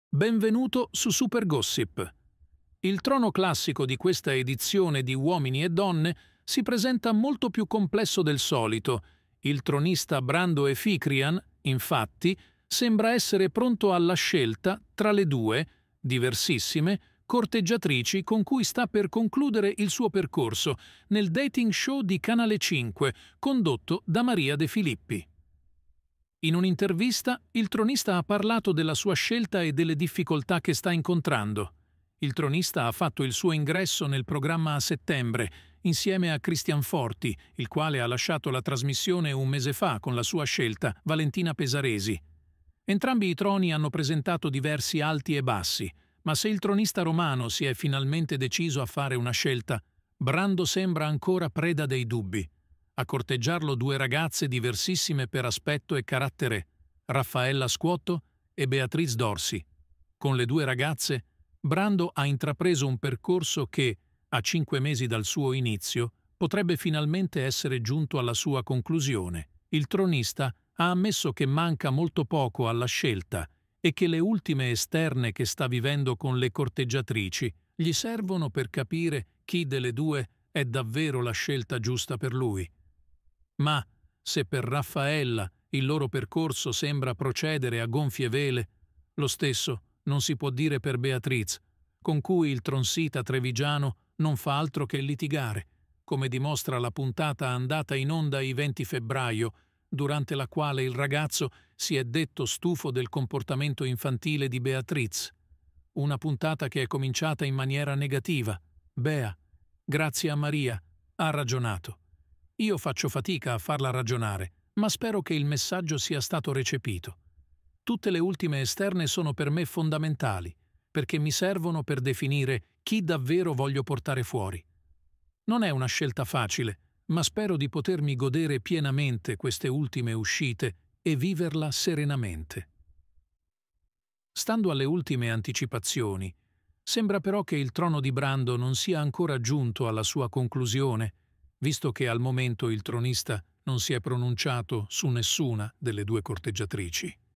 ❓❓💥⛔️💣SULLA SUA SCELTA A UOMINI E DONNE BRANDO EPHIKRIAN GRAZIE A MARIA DE FILIPPI FINALMENTE...
Il tronista ha ammesso che manca molto poco alla scelta e che le ultime esterne che sta vivendo con le corteggiatrici gli servono per capire chi delle due è davvero la scelta giusta per lui. Ma, se per Raffaella il loro percorso sembra procedere a gonfie vele, lo stesso non si può dire per Beatriz,